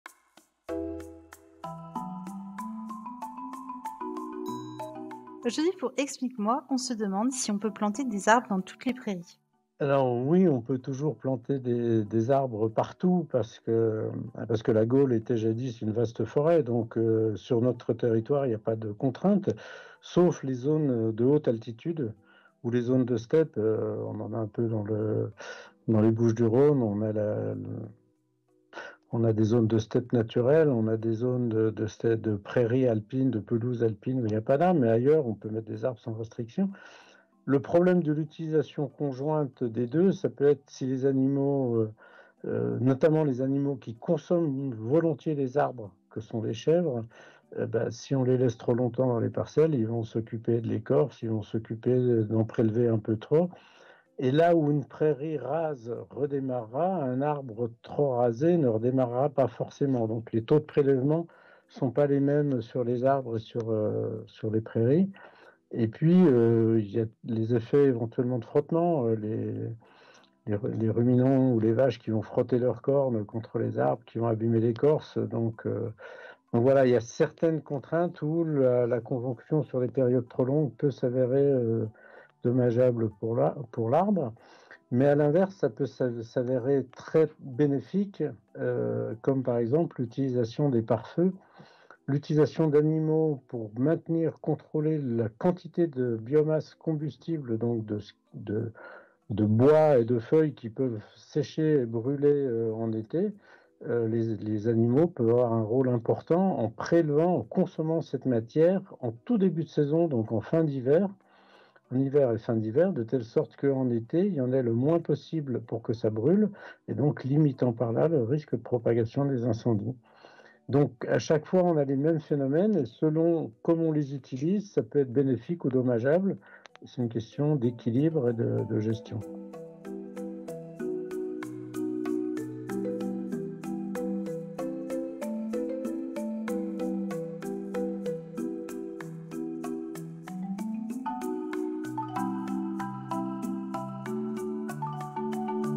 Explique moi l’agroforesterie Ep 3 Peut-on planter des arbres partout ? Avec Gilles Brunschwig
Dans les précédents épisodes, nous avons vu que les arbres avaient de nombreux avantages pour la planète et pour les éleveurs et leurs animaux. Mais est-ce vraiment une bonne idée de transformer les prairies en forêts? Voici le dernier épisode de notre série sur l'agroforesterie avec Gilles Brunschwig, professeur à Vetagro Sup.